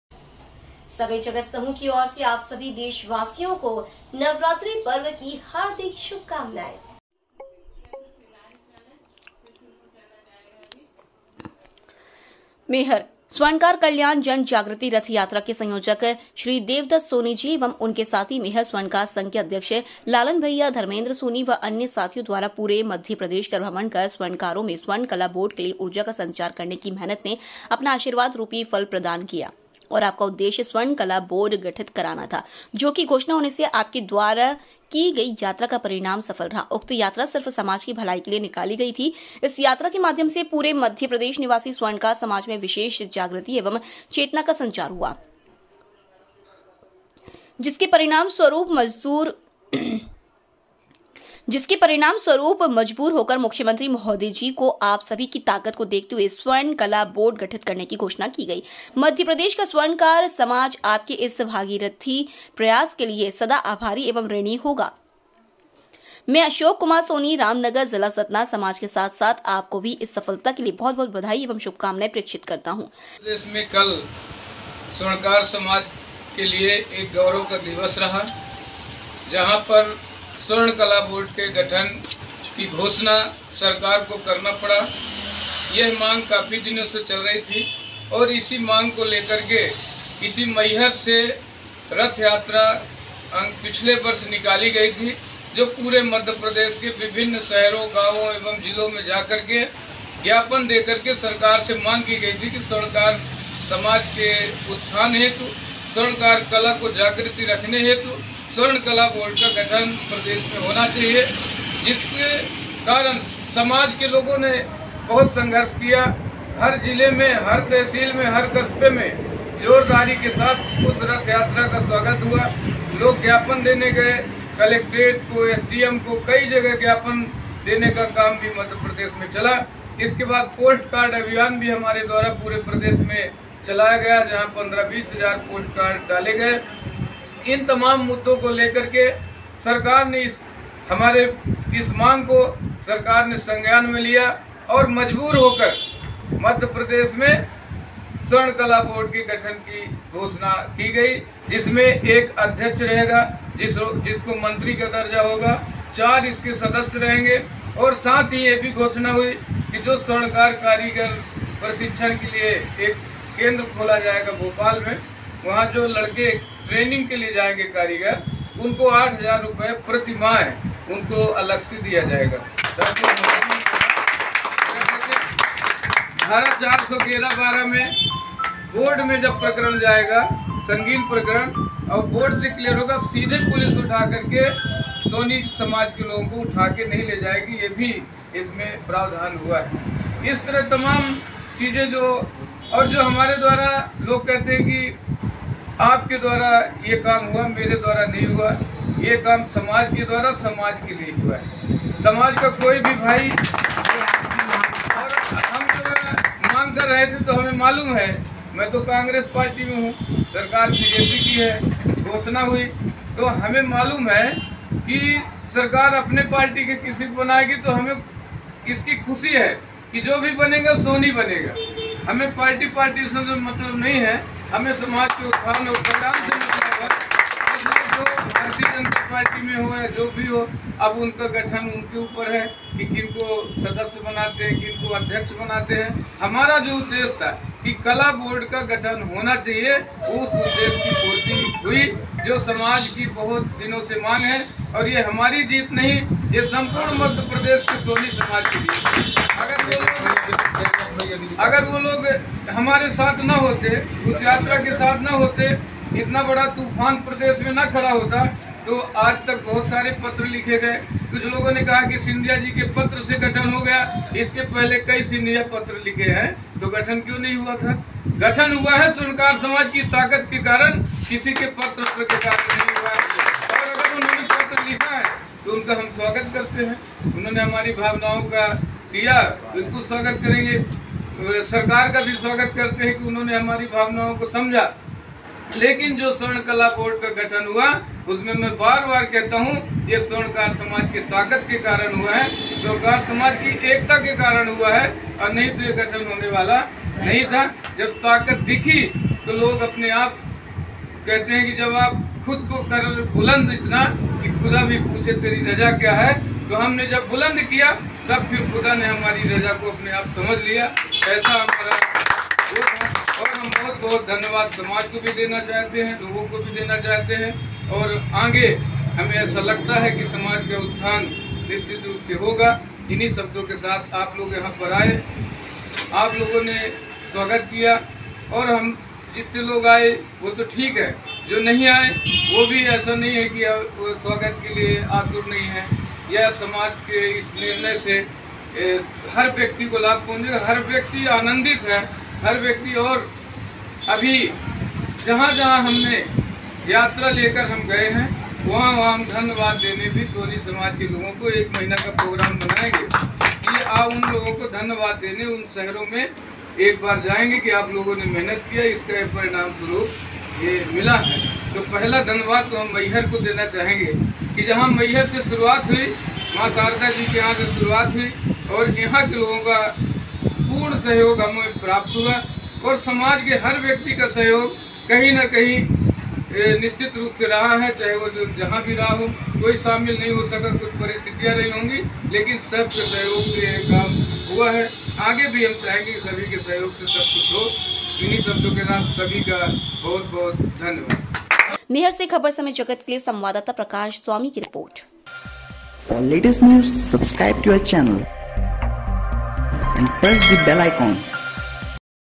मेहर | स्वर्णकार कल्याण जनजागृति रथ यात्रा के संयोजक श्री देवदत्त सोनी जी एवं उनके साथी मैहर
मेहर संवाददाता प्रकाश स्वामी की रिपोर्ट